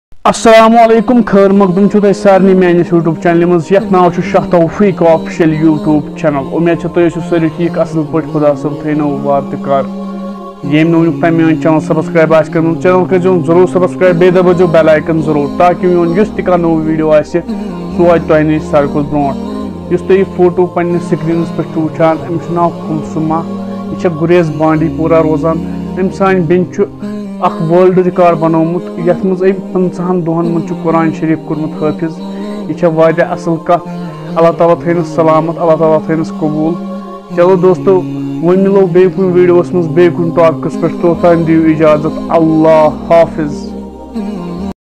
mashallah a young girl kulsuma created a world record by becoming a hifze quran in just 25 days
mashallah a young girl kulsuma bint majeed from gurez bandipora created a world record by becoming a hifze quran in just 25 days